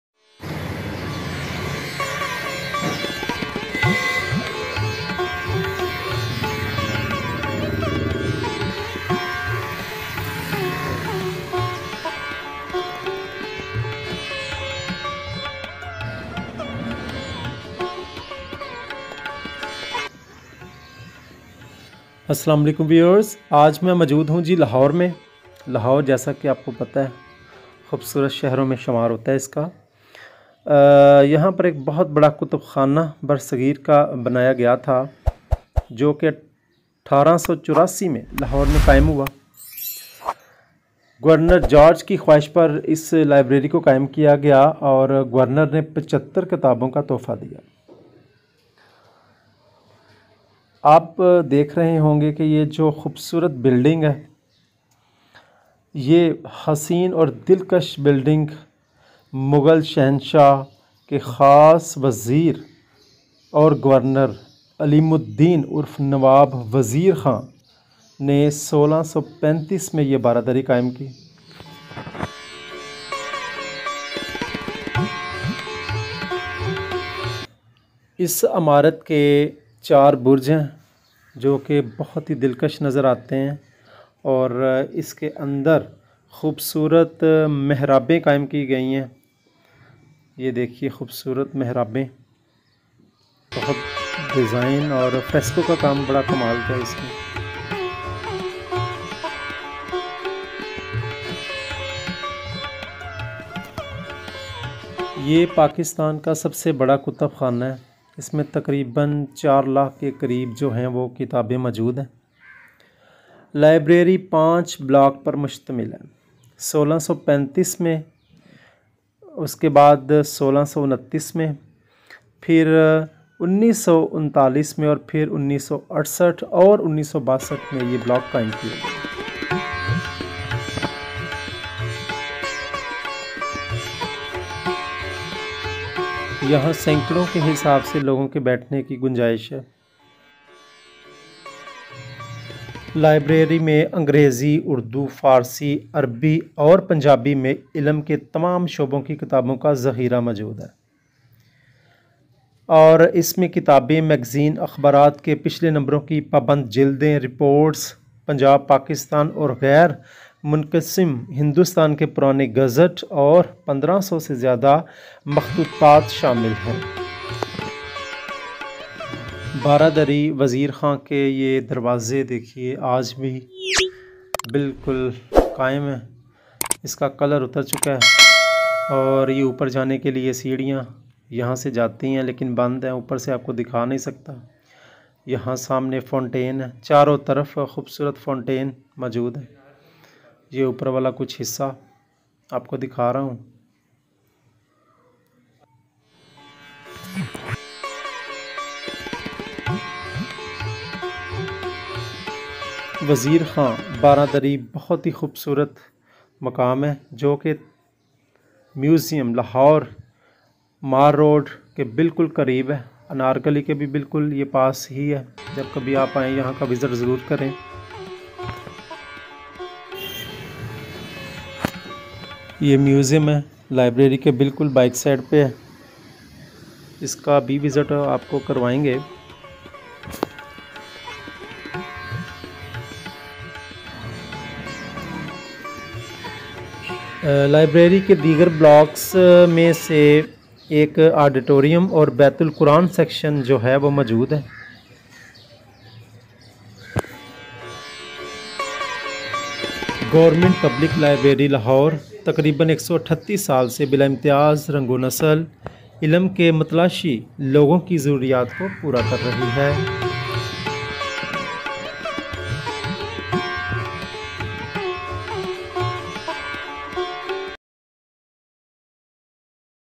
Punjab Public Library Lahore 1884 |VS | Bara Dari Wazir Khan 1635 | Documentary History |Urdu/Hindi
Aslam o alekm
Punjab Public Library Lahore 1884
Bara Dari Wazir Khan 1635
The library began operations in the Baradari Wazir Khan, an imperial building built by Nawab Wazir Khan, the governor of Lahore, during Emperor Shahjahan’s reign with the assistance of the government of the Punjab. Its purpose is to be a public library and reading room.
Other library blocks were built in 1939, and a block housing the auditorium and Bait-ul-Quran section was constructed in 1968 and inaugurated by General Mohammad Musa, then governor of West Pakistan.
Govt. Punjab Public Library, Lahore, lies in the heart of the city of Lahore at Library Road near the Lahore museum, off Shahrah-e-Quaid-e-Azam. It is surrounded by educational institutions and the Punjab University (Old Campus). The commercial centre of Anarkali, the main offices of the Metropolitan Corporation Lahore (Town Hall) and the provincial government offices, i.e., the Civil Secretariat, are a short distance from building.
The library holds a collection of books in all fields of knowledge in English, Urdu, Persian, Arabic and Punjabi. Its total collection is about 0.3 million volumes and includes books, bound volumes of back numbers of magazines/ newspapers, reports, the old gazettes of Punjab, Pakistan and undivided India, and more than 1500 manuscripts. The library receives 170 magazines, 24 journals by subscription and the rest as free copies. 12 dailies are received in the library.
must watch